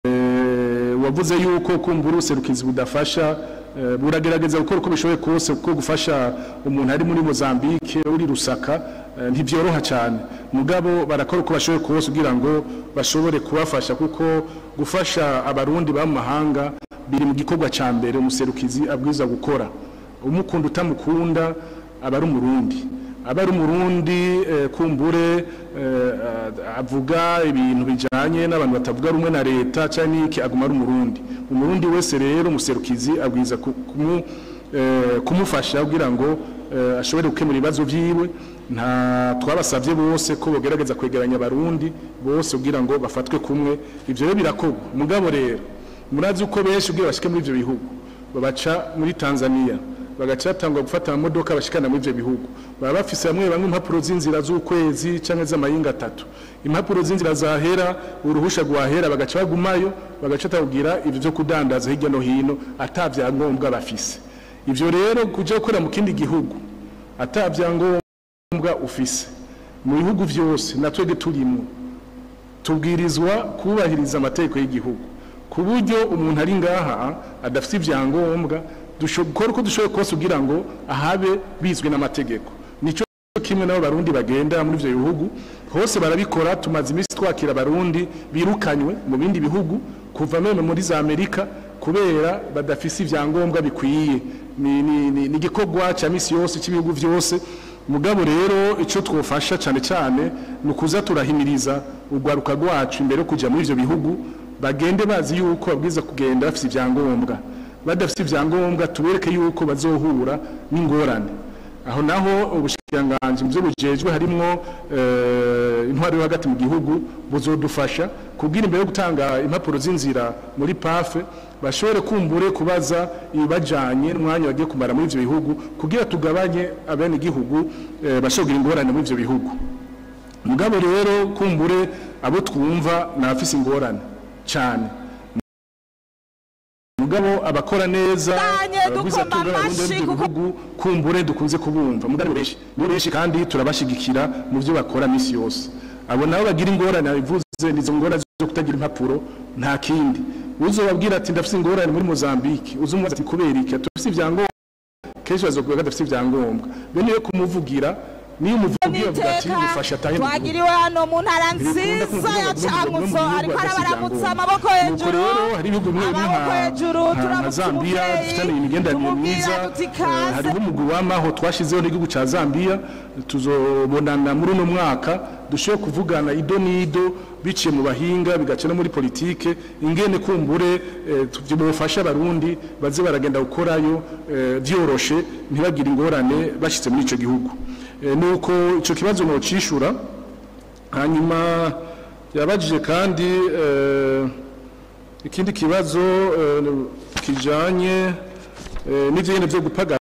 Kuki ama ambassades adafasha abarundi bari mu mahanga bab bafise ingorane? Zambia, Mozambique, ...
Welcome to JUMUIYA TV: Live Broadcasting and TV Shows on Social Media and TV in Burundi and EAC